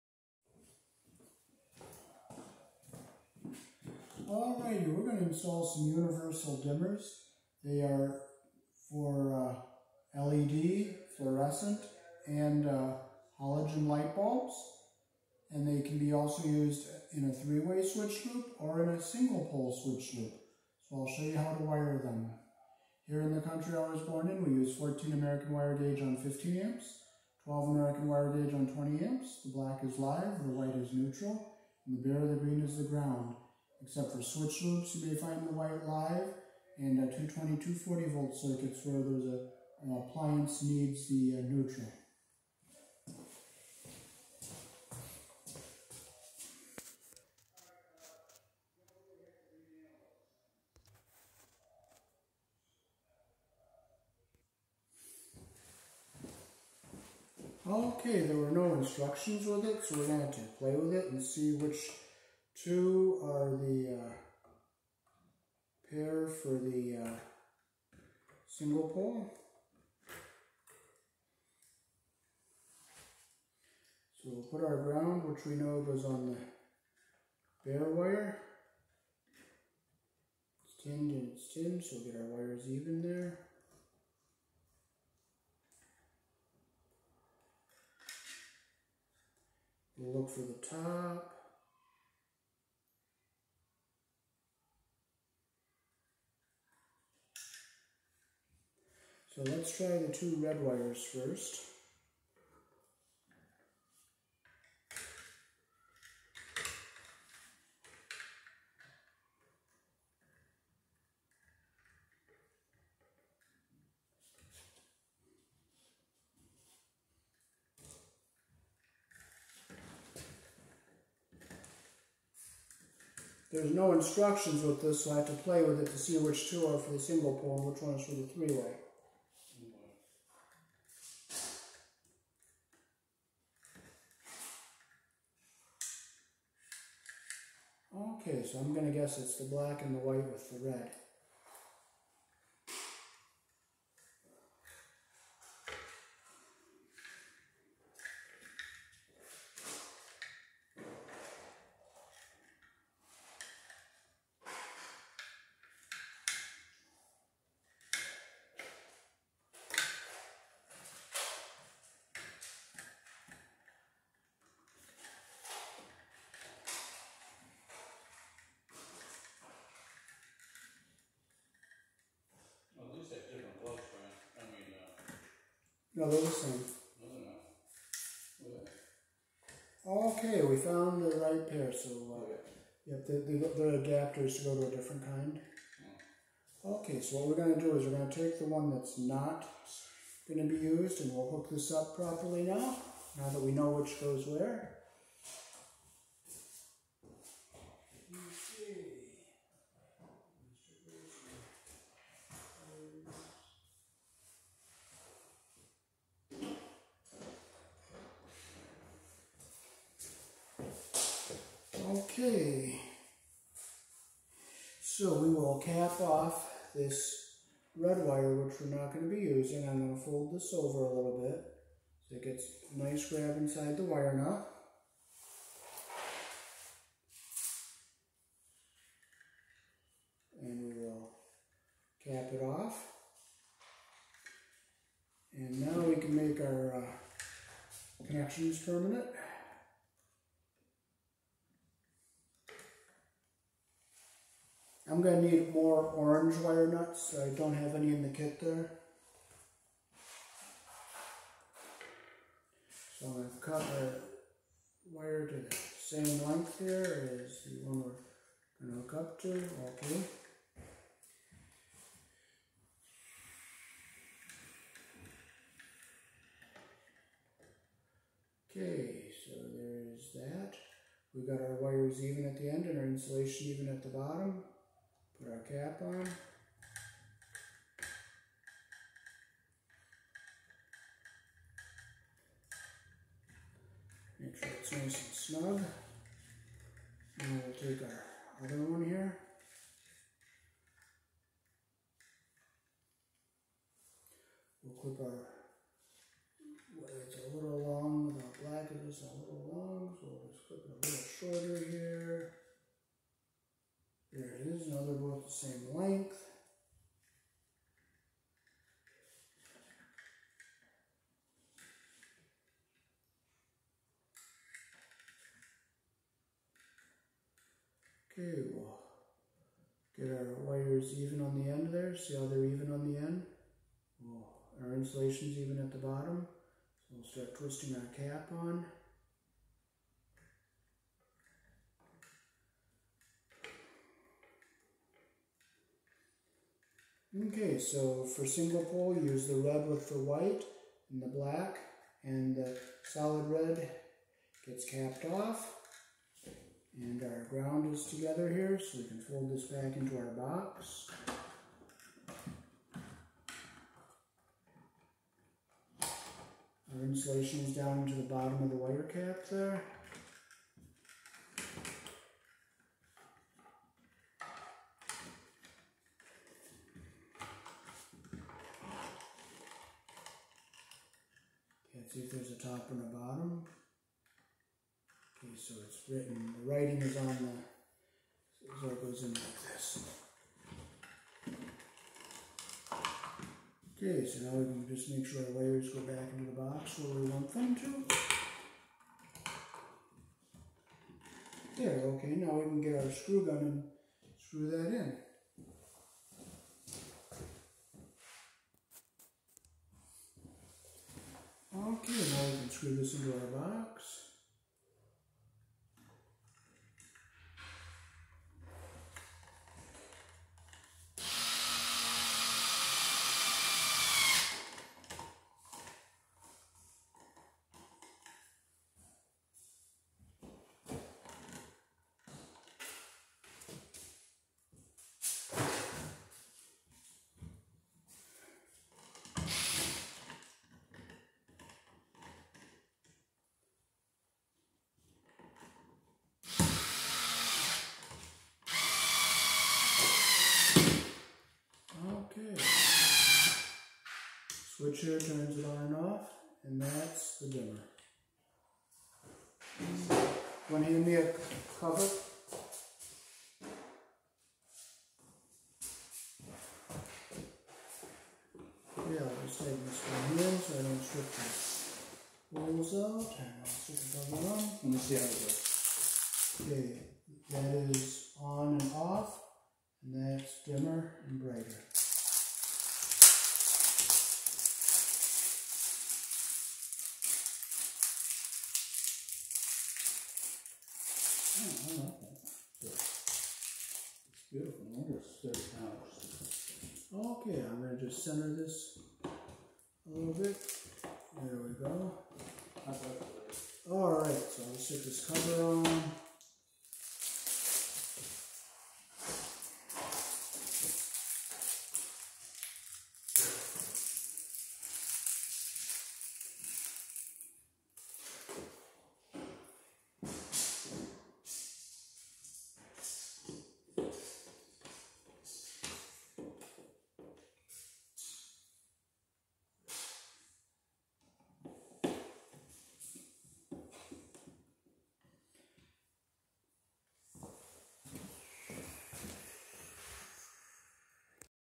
how to wire a universal dimmer 3 way or single pole
how to wire a dimmer  consult with codes before doing electrical work